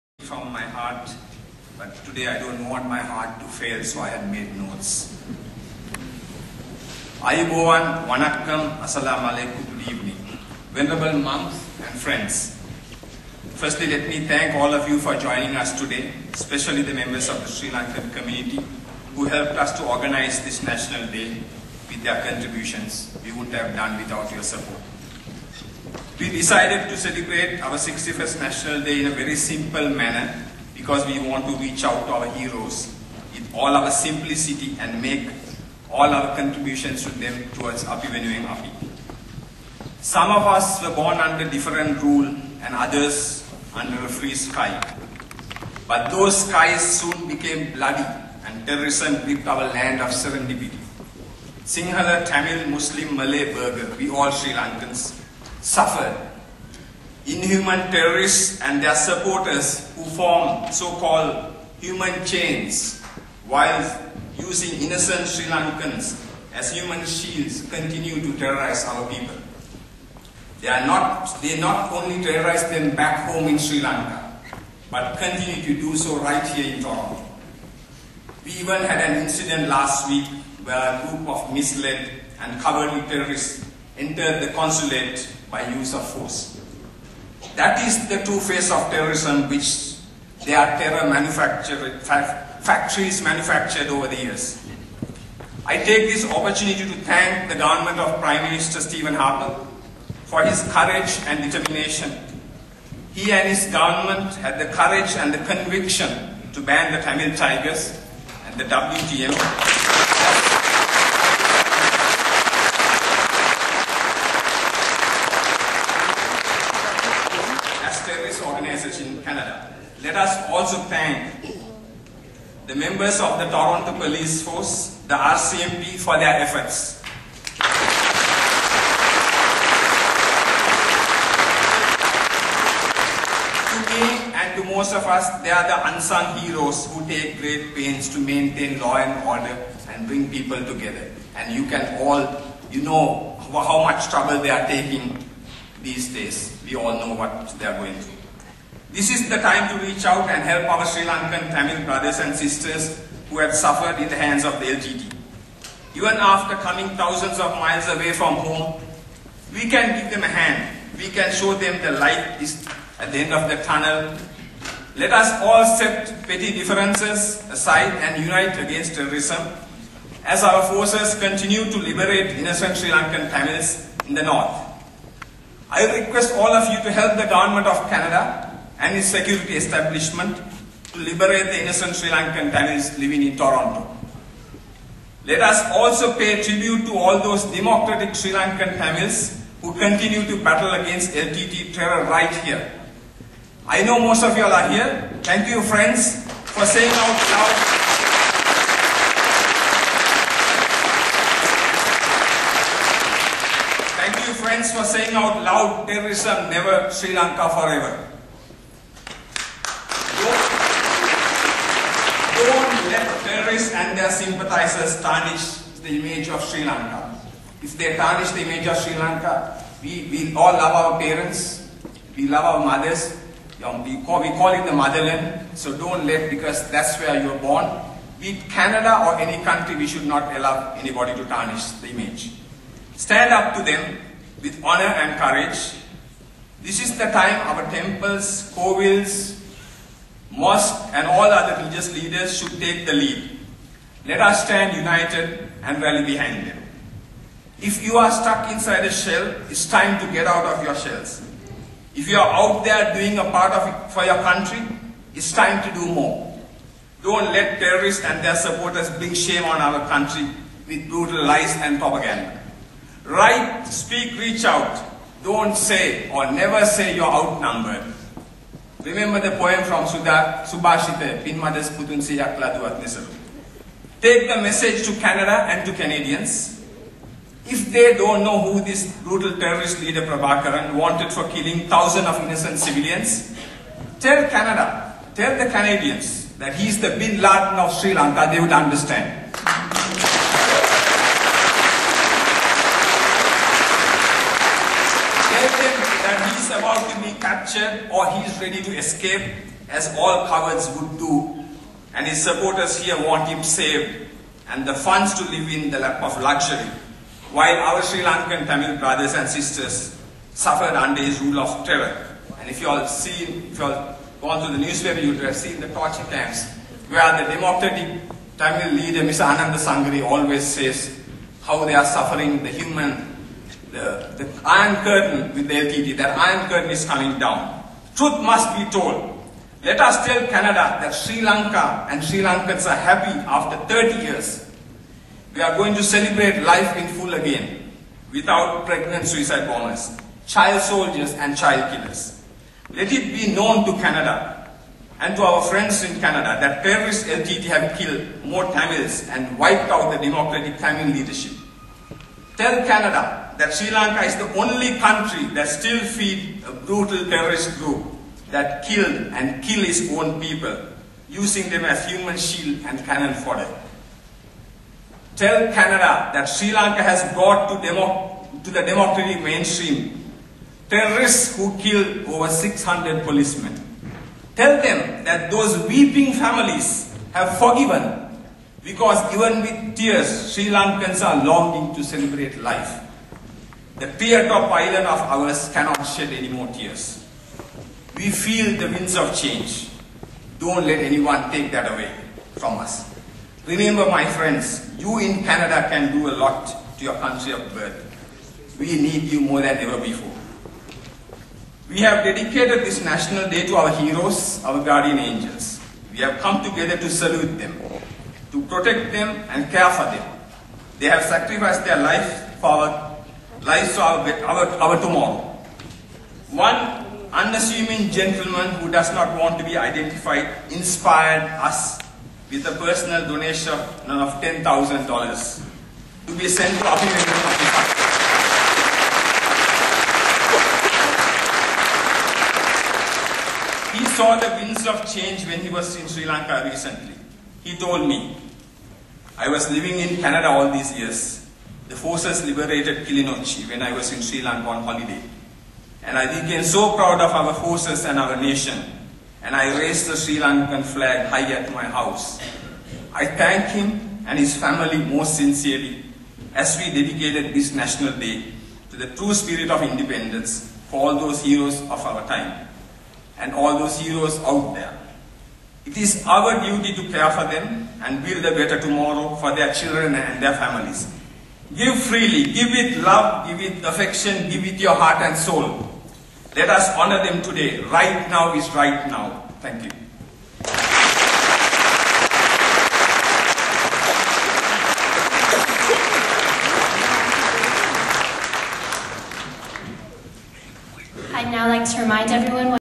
Speech by Sri Lankan Consulate General Bandula Jayasekara on the 61st Independance day celebration
On February 4th, 2009 Sri Lanka held its 61th Independance day celebration. In Toronto, Canada, the cosulate general Bandula Jayasekara gave a good speech at the Independance day celebration held at the Japanese cultural center. He pointed out that all Sri Lankans should get together and support their mother land Sri Lanka at this moment where its looking for a new begining as it is on the verge of defeating the Tamil Tiger rebels who created terror for the last 26.